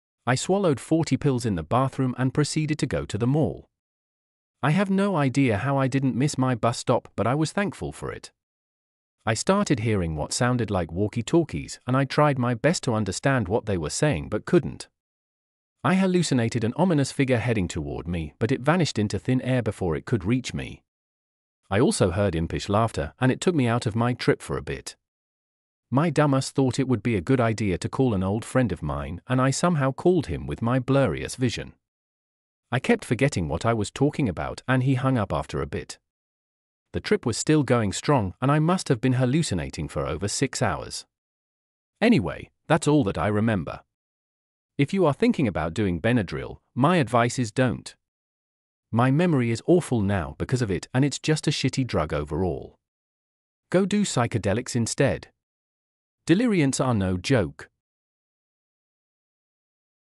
Diphenhydramine Trip Report 2: Tripping at the Mall - 1000 mg
This is about the time I took forty pills of DPH and thought it would be wise to go to the mall. I have no idea how I didn't have a seizure but don't do anything like what I did. I was young and stupid.